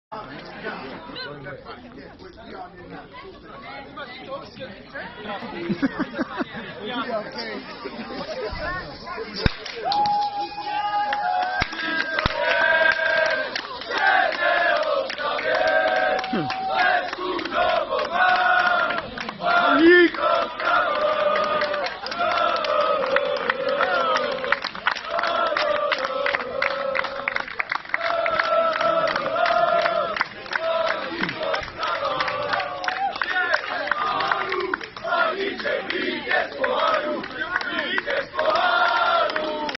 Oslava vítězství Memoriálu Evžena Hadamczika (2018)
Turnajové prvenství zůstalo v Ostravě zásluhou našeho týmu kategorie U11. Oslava s fanoušky nesměla chybět.